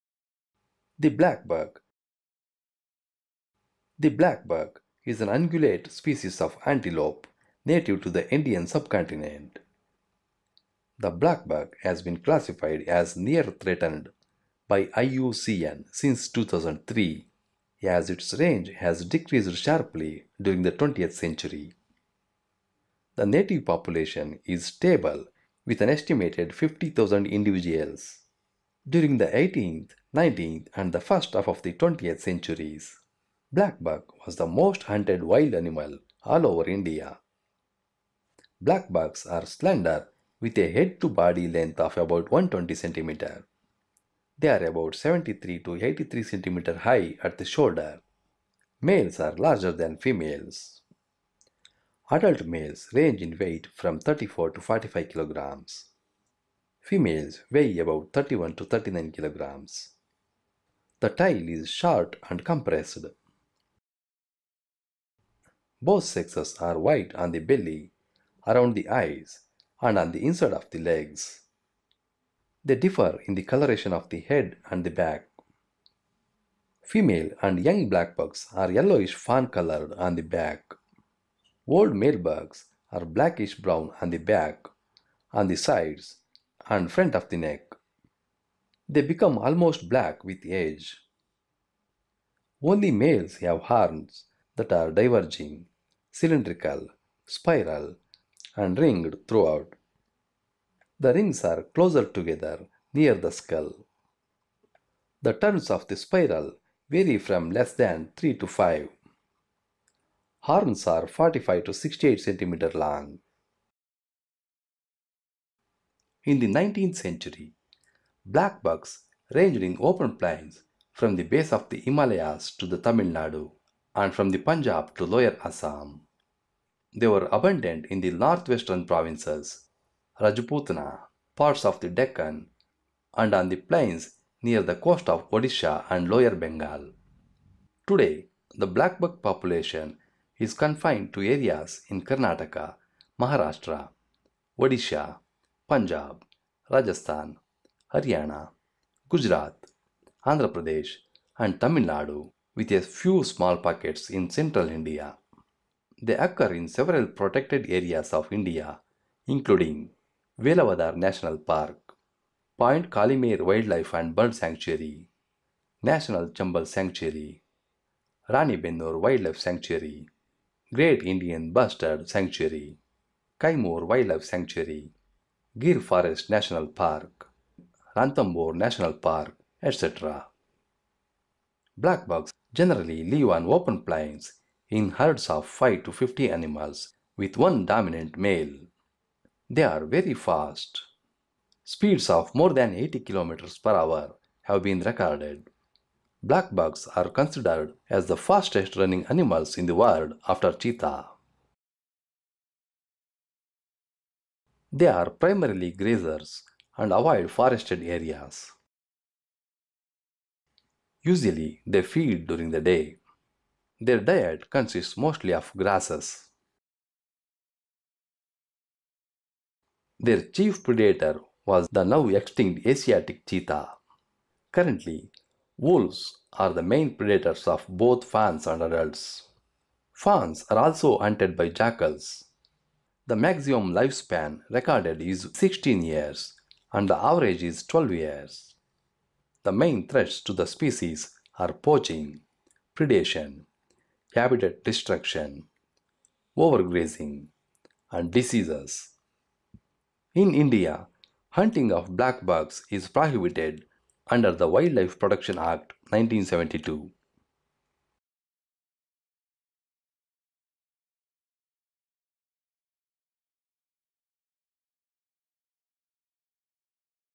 Fastest running Indian animal Blackbuck
The Blackbuck is an ungulate species of antelope native to the Indian subcontinent.

He has it's range has decreased sharply during the 20th century, the native population is stable with an estimated 50,000 individuals during the 18th, 19th and the first half of the 20th centuries. Blackbuck was the most hunted wild animal all over India.

Blackbucks are slender with head to body length of about 120 centimeter. They are about 73 to 83 centimeteres high at the shoulder, males are larger than females, adult males range in weight from 34 to 45 kilograms females vary about 31 to 39 kilograms the time is short and compressed.

Both sexes are white on the belly , around the eyes and on the inside of the legs, they differ in the coloration of the head and the back female and young Blackbucks has yellow color on the back. Old male Bucks are blackish brown on the back, on the sides and on the front of the neck.